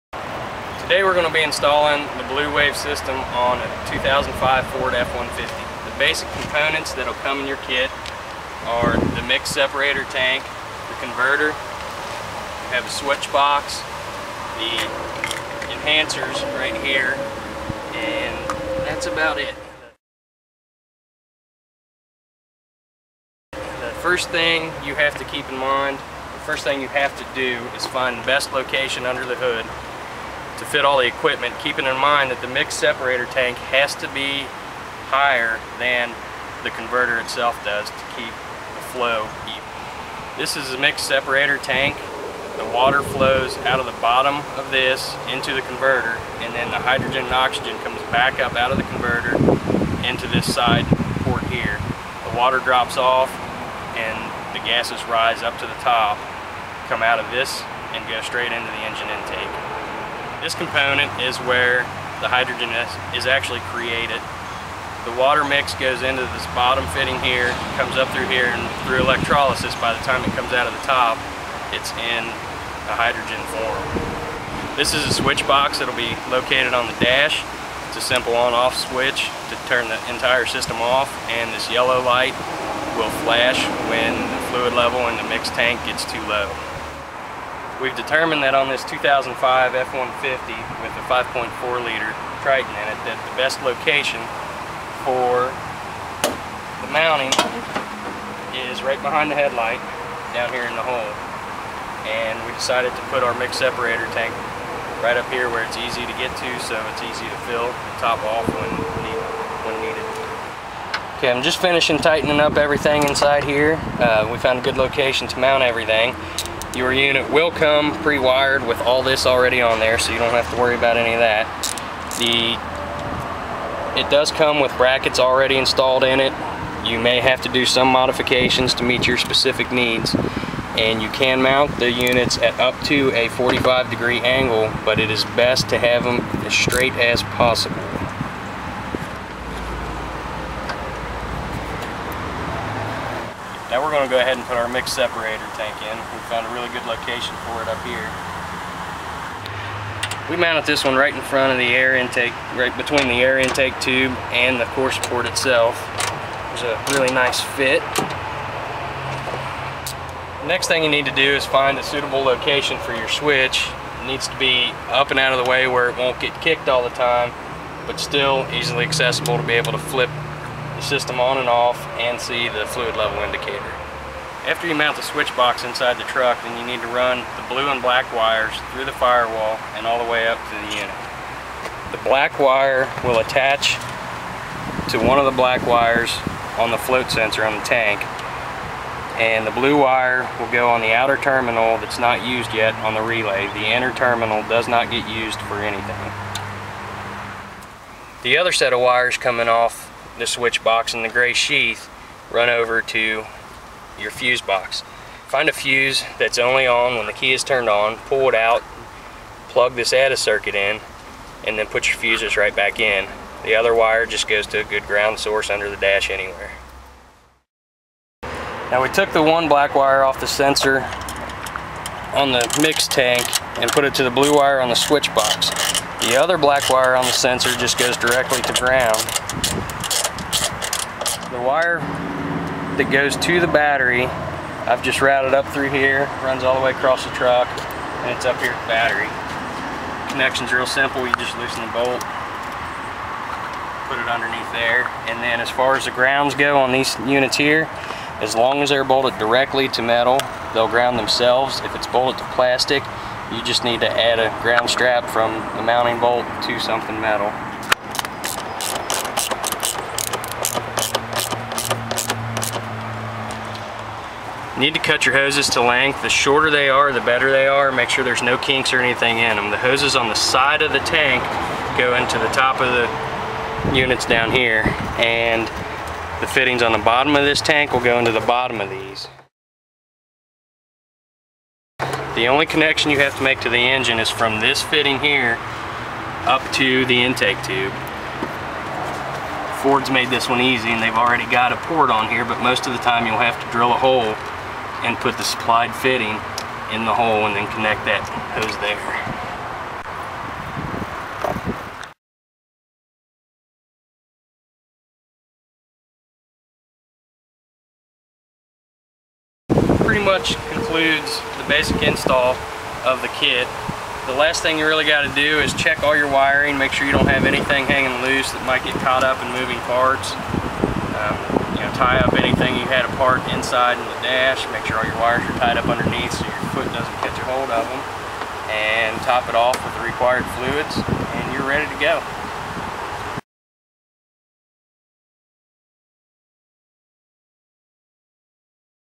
Hydrogen On Demand Installation Video
How to install the BluWave Hybrid Conversion system. Its Hydrogen on Demand.